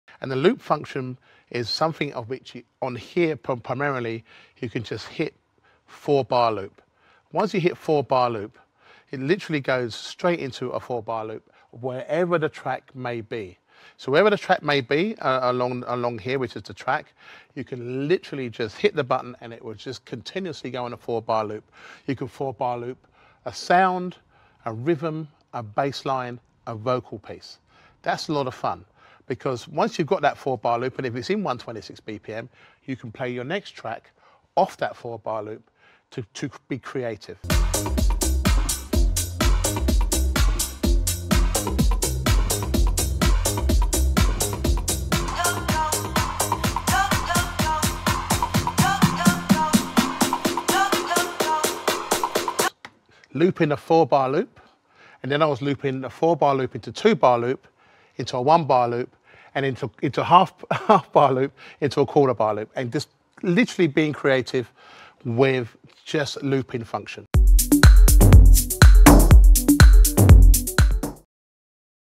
Carl Cox - What Is The Loop Function?
What is the 'Loop' function? 🤔
In this beginner lesson preview, the master@CarlCoxofficialTV explains the basics of the 'Loop' function and how you can use the shortcuts to edit a track into a 4-bar, 2-bar, 1-bar, 1/2, and finally into 1/4 loop. A unique way to mix different elements of the track 🎵 Join now to view all our lesson content and more!